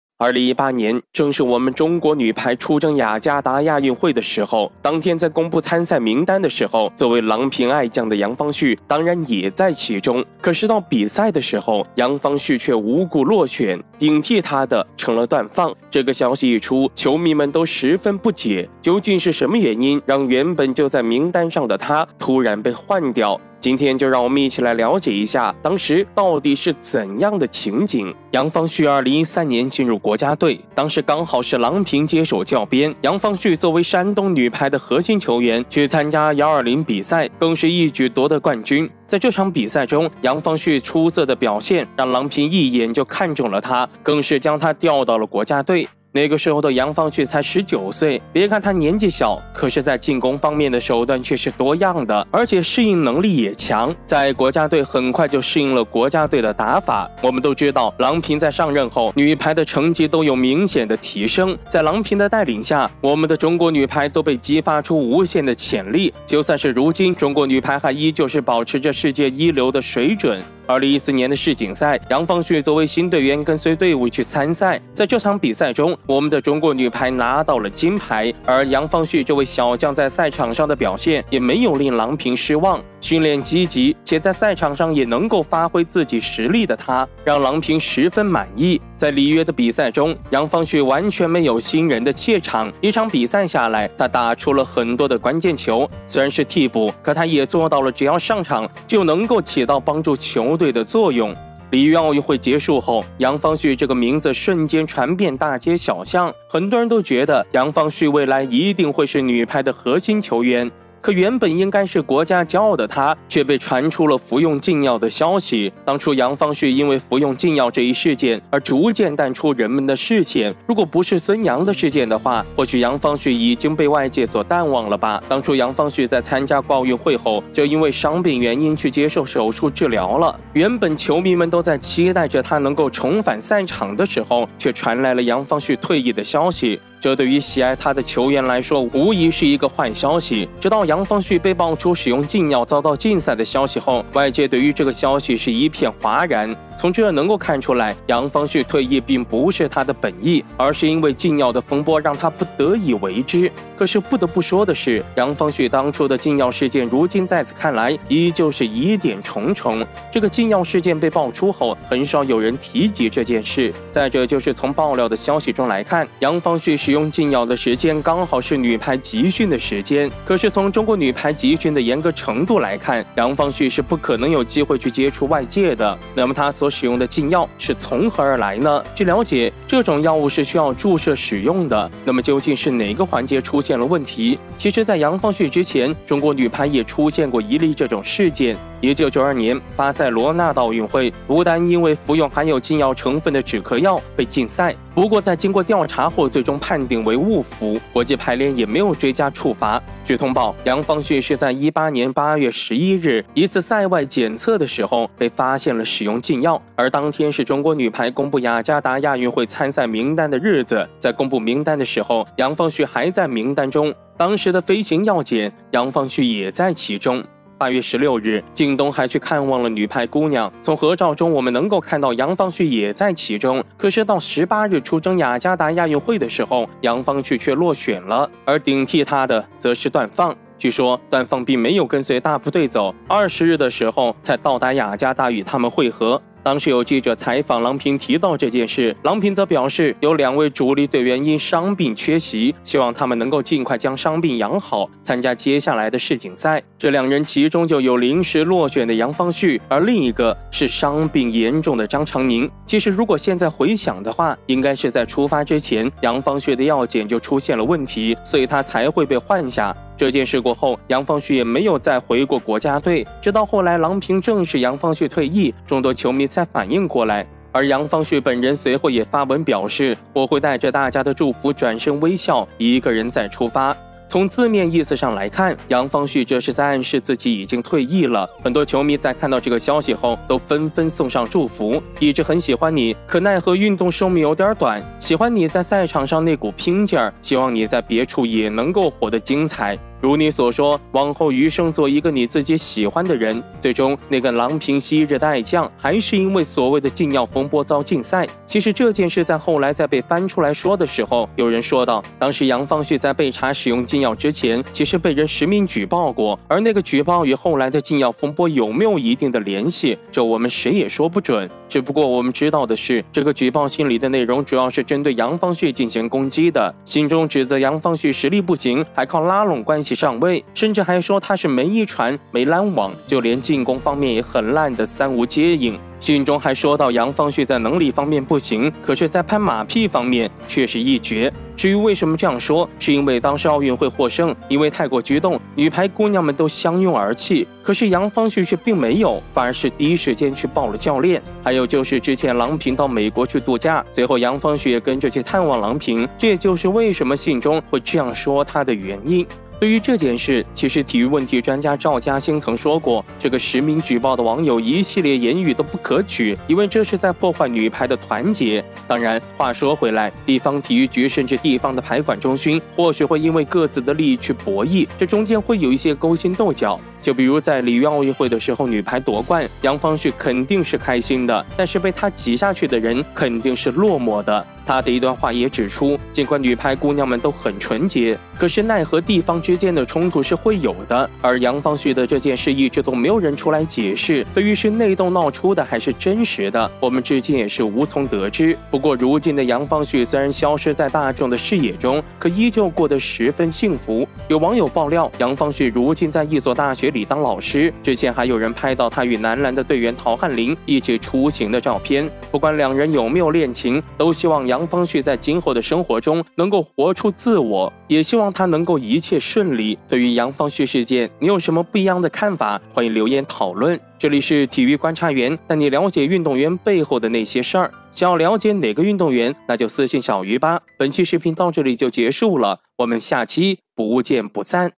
杨方旭事件疑团重重，郎平为何一直秘而不宣？【体娱一品官】
2018年正是中国女排出征雅加达奥运会的时候，当天在公布参赛名单的时候，作为郎平爱将的杨方旭当然也在其中，可是到比赛的时候杨方旭却无故落选，顶替她的成了段放。这个消息一出球迷都十分不解。究竟是什么原因，让原本在名单上的他忽然被换掉。
体娱一品官 杨方旭 郎平 我们的内容专注于体育赛事，绝对不会包含任何暴力或血腥元素。